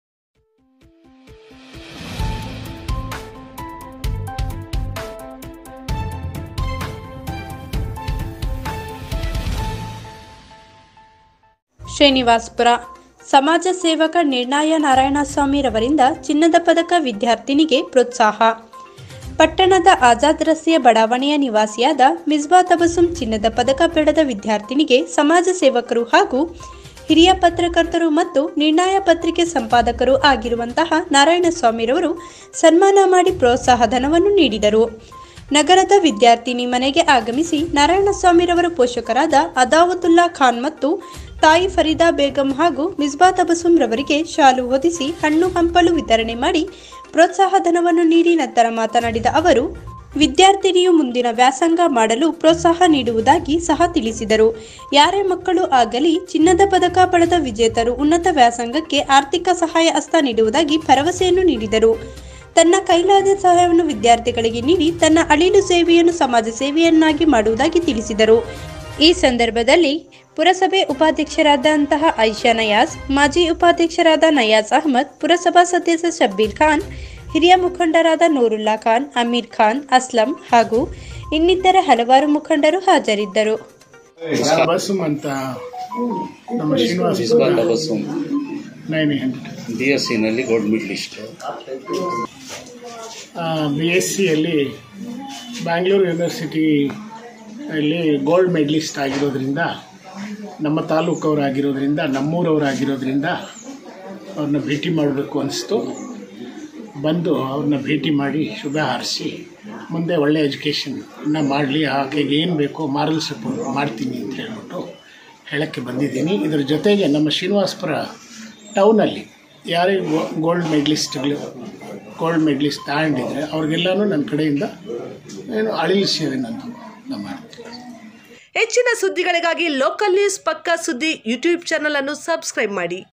ಚಿನ್ನದ ಪದಕ ಪಡೆದ ವಿದ್ಯಾರ್ಥಿನಿ ಮಿಸ್ಬಾ ತಬಸ್ಸುಮ್ ಸನ್ಮಾನಿಸಿ ಪ್ರೋತ್ಸಾಹ ಧನವನ್ನು ನೀಡಿದ ನಿರ್ಣಯ ನಾರಾಯಣಸ್ವಾಮಿ
Srinavaspur ನಗರದ ಚಿನ್ನದ ಪದಕ ಪಡೆದಂತಹ ವಿದ್ಯಾರ್ಥಿನಿ ಮಿಸ್ಬಾ ತಬಸ್ಸುಮ್ ಸನ್ಮಾನಿಸಿ ಪ್ರೋತ್ಸಾಹ ಧನವನ್ನು ನೀಡಿದ ನಿರ್ಣಯ ನಾರಾಯಣಸ್ವಾಮಿ